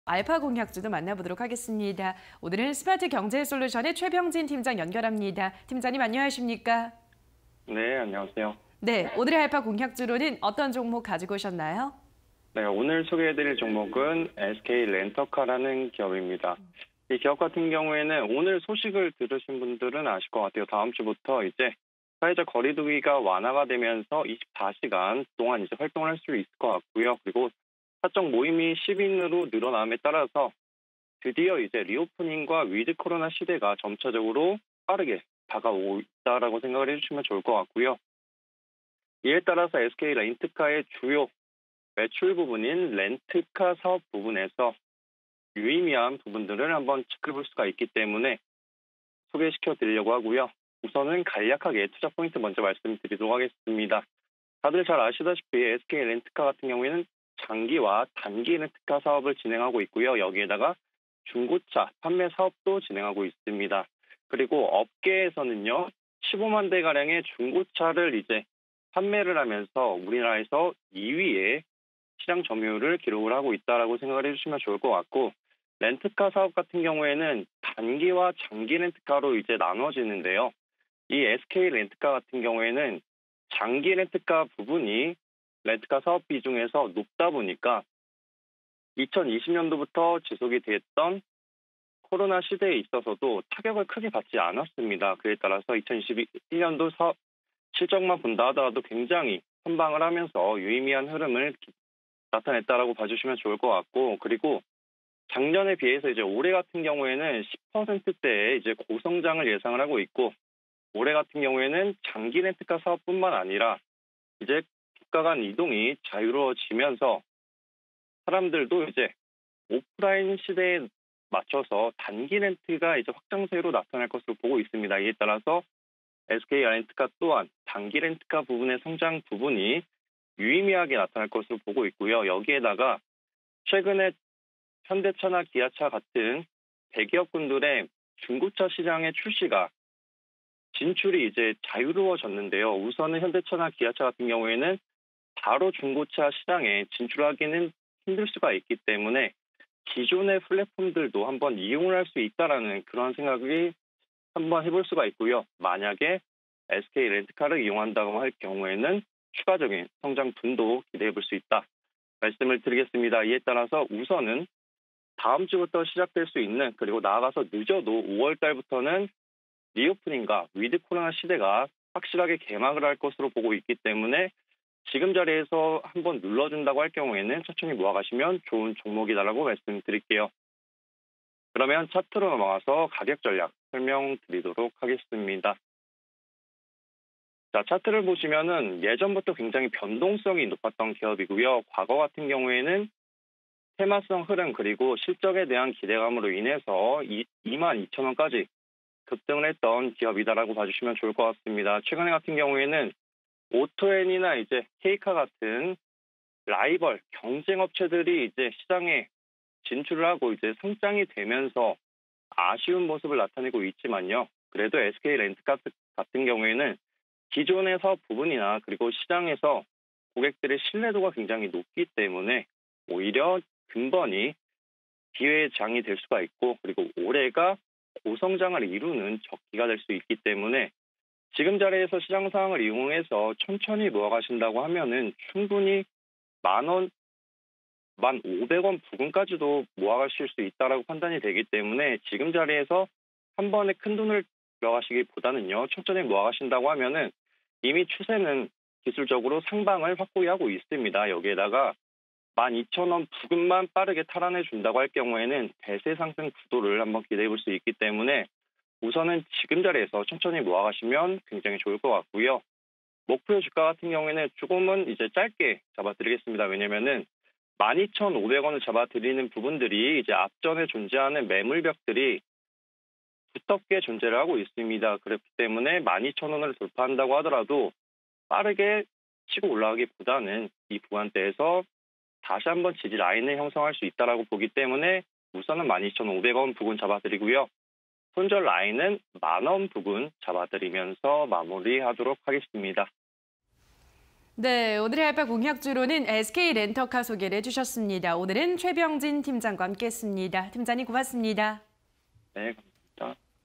SK렌터카(068400)... 렌탈 매출 72.7%·중고차 매출 24.1%_알파 공략주 (20220329)
SK렌터카(068400)
- 렌탈 매출 72.7%·중고차 매출 24.1%
- 렌터카 및 모빌리티 산업에 적극 대응
- 시장 성장 속 신규 고객 창출 지속
- 기존 플랫폼 활용한 꾸준한 성장 기대
- 리오프닝 수혜 관점에서 접근